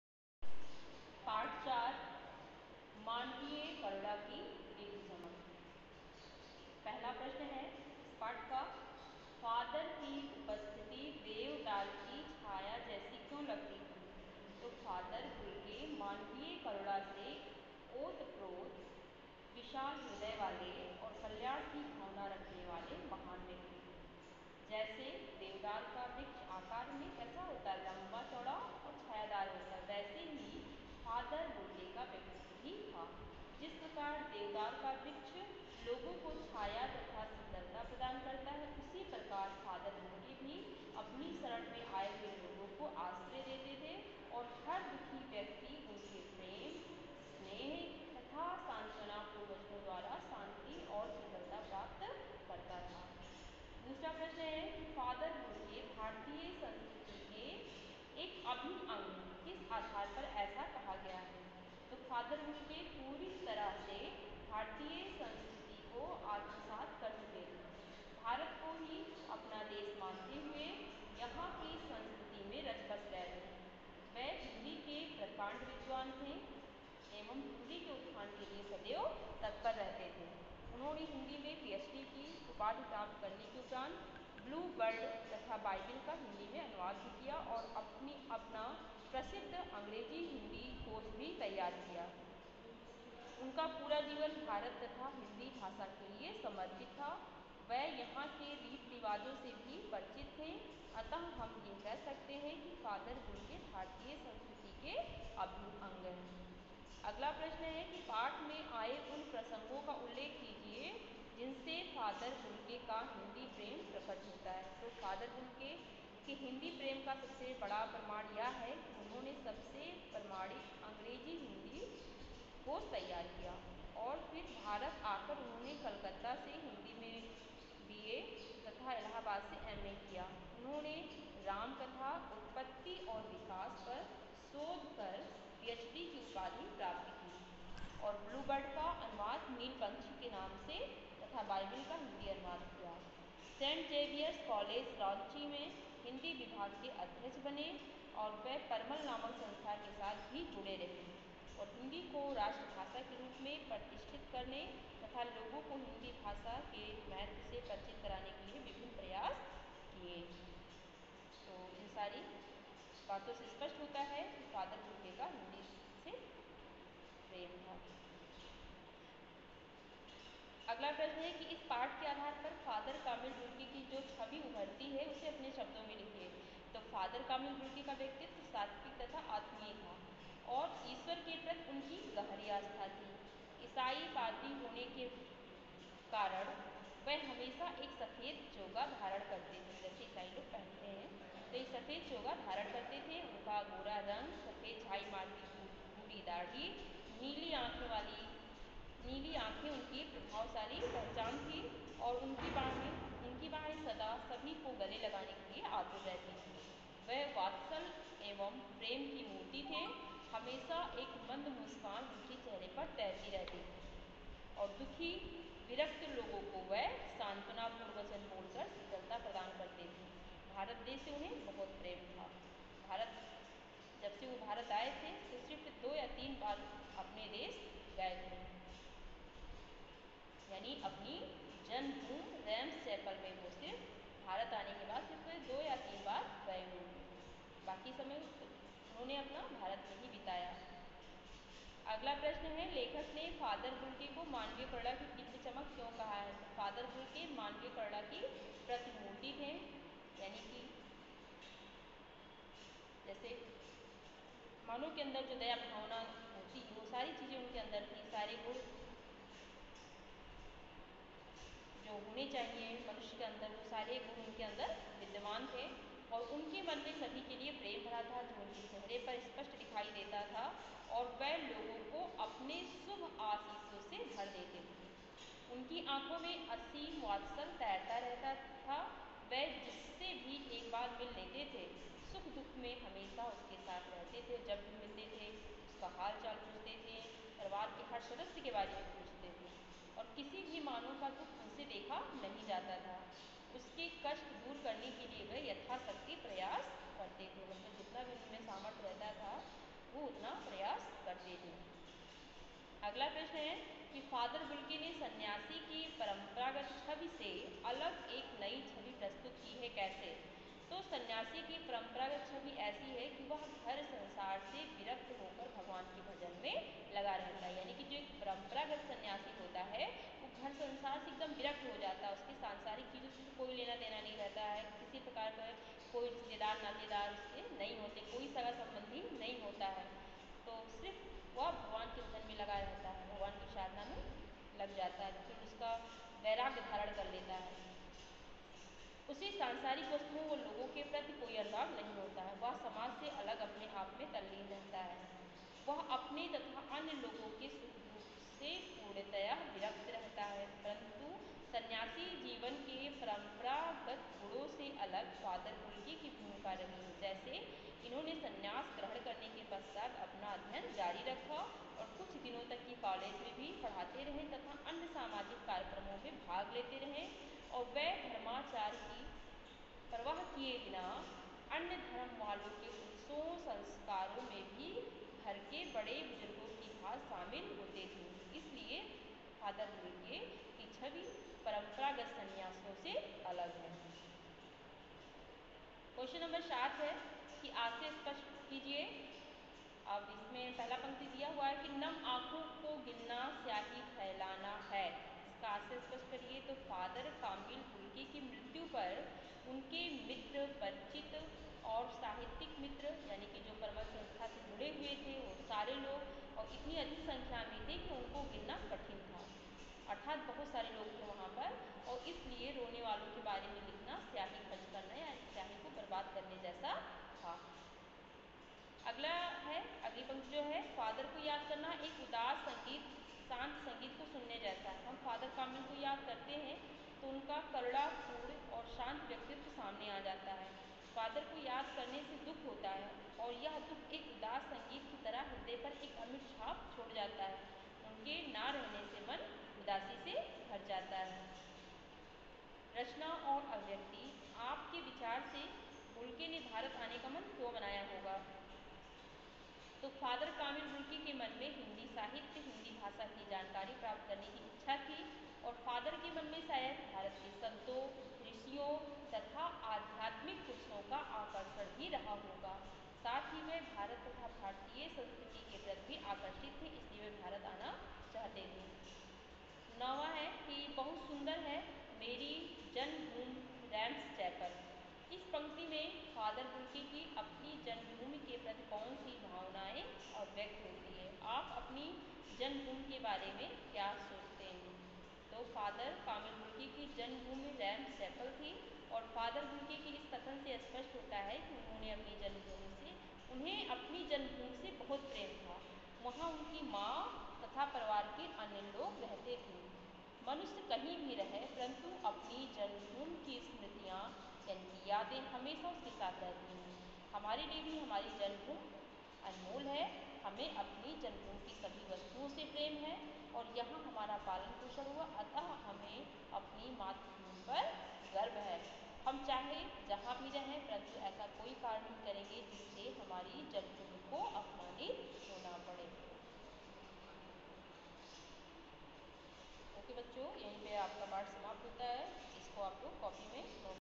August 25, 2020
कक्षा १०पाठ मानवीय करुणा की दिव्य चमक का प्रश्नोत्तर अभ्यास